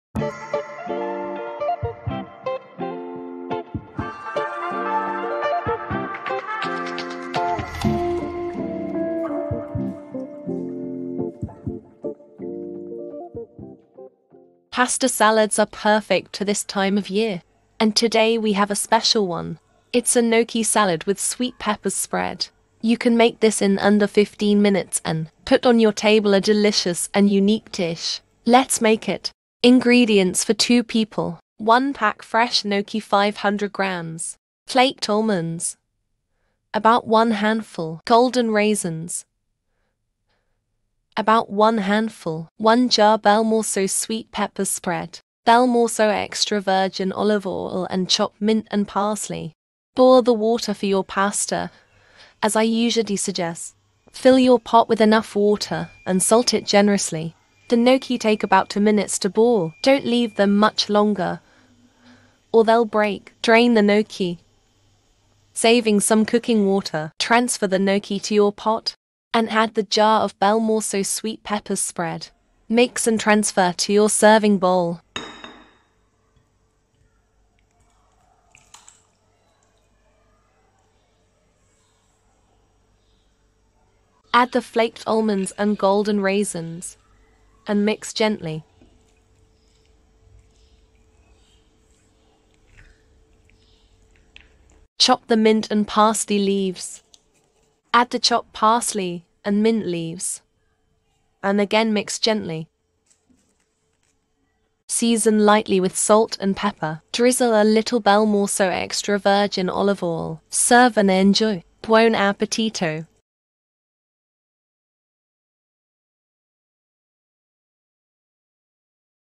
Gnocchi Salad with Sweet Peppers
If you're looking for a flavorful pasta salad, it’s Gnocchi Salad with Sweet Peppers Spread. You can make this in under 15 minutes and put on your table a delicious and unique dish. Let’s make it.

♦ 1 pack fresh gnocchi 500g (don’t be fooled by the grams as gnocchi are much heavier than other pasta)
♦ 1 jar Belmorso Sweet Peppers Spread
♦ flaked almonds – about 1 handful
♦ golden raisins – about one handful
♦ salt and pepper
♦ Belmorso extra virgin olive oil
♦ chopped mint and parsley

Quick and easy to prepare, this dish can be served in a pretty bowl to impress unexpected guests. You can easily make this dish if you keep some gnocchi and jars of Belmorso Sweet Peppers Spread on hand; you probably already have the rest of the ingredients.

Facebook and Instagram: @tastewithgusto
Be one of the first subscribers to get the latest recipes and offers.
I hope you love this recipe.
Buon Appetito!💚